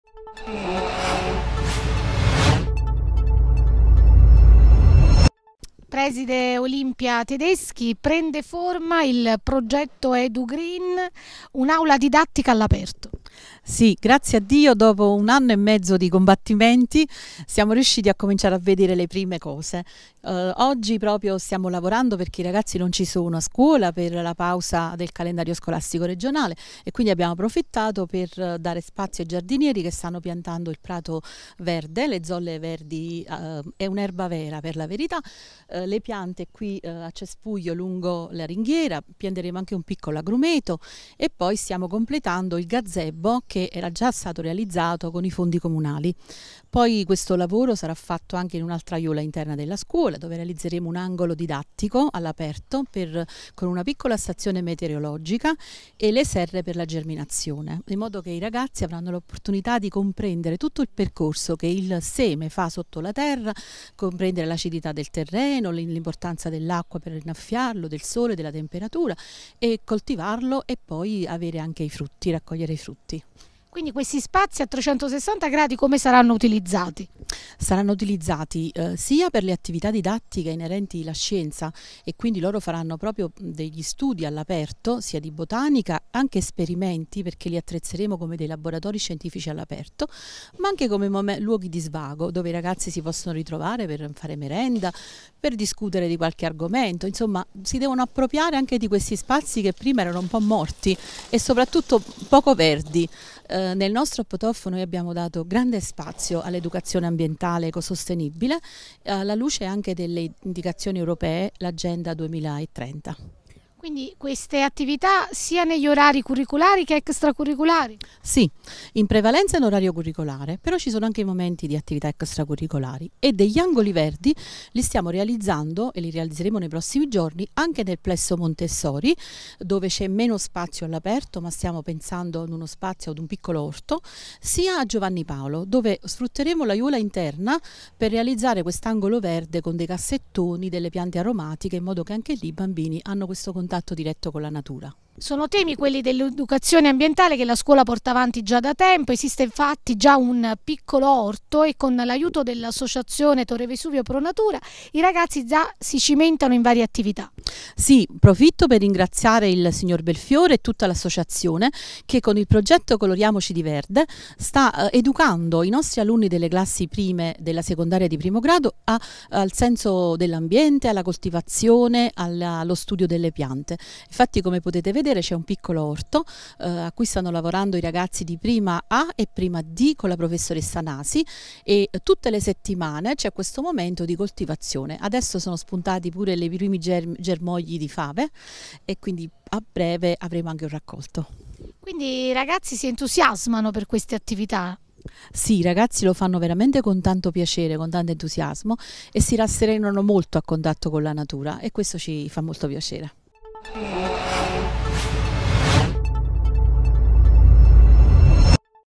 Edugreen: prende forma il progetto all'Istituto Giacomo Leopardi
Sono partiti in questi giorni i lavori negli spazi all'aperto dell'Istituto Giacomo Leopardi di Torre del Greco, nell'ambito del progetto Edugreen, fortemente voluto dalla scuola.

Sarà una vera e propria area didattica all'aperto.

La scuola porta avanti da tempo i tempi dell'educazione ambientale ecosostenibile.

Ai microfoni di TVCITY, la dirigente scolastica Olimpia Tedeschi spiega le finalità del progetto e le attività che verrano realizzate.